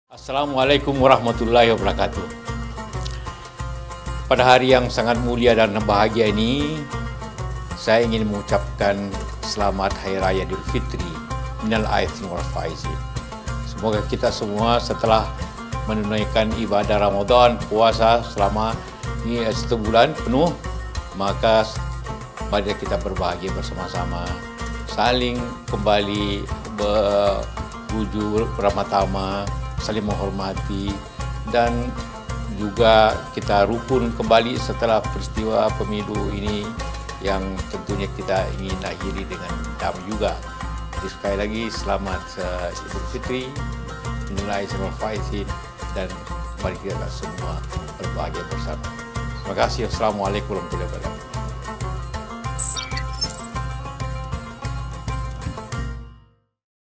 Ucapan Idulfitri 1440 H dari Jusuf Kalla | Medcom ID
Wakil Presiden Republik Indonesia, H. M. Jusuf Kalla, mengucapkan Selamat Hari Raya Idulfitri 1440 H. Semoga seluruh warga Indonesia bisa berbahagia bersama-sama, kembali saling menghormati dan rukun setelah pemilu, serta damai selalu.